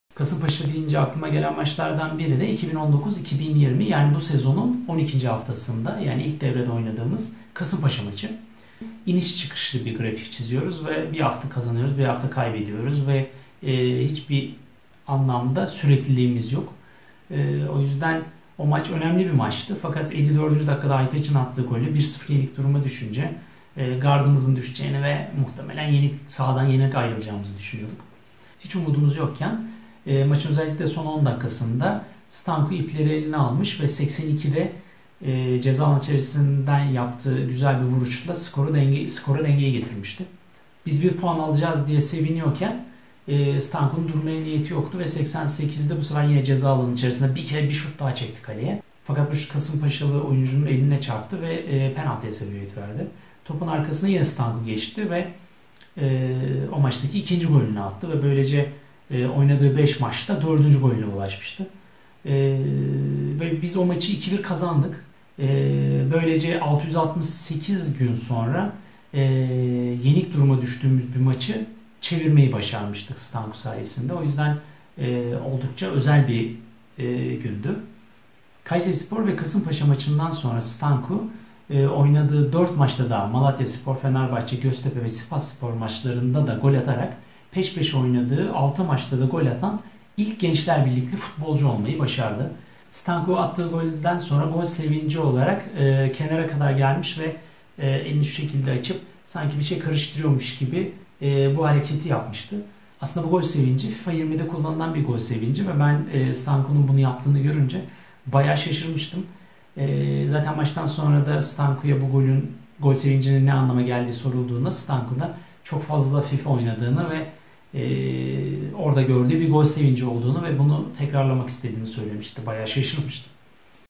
"668 Gün Sonra" Kasımpaşa 1-2 Gençlerbirliği Maç Anısı - 24.11.2019 - 2019-2020 Sezonu 12. Hafta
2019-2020 Sezonu Spor Toto Süper Lig Cemil Usta Sezonu 12. Hafta Maçı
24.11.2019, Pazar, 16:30
Recep Tayyip Erdoğan Stadyumu
İstanbul, Türkiye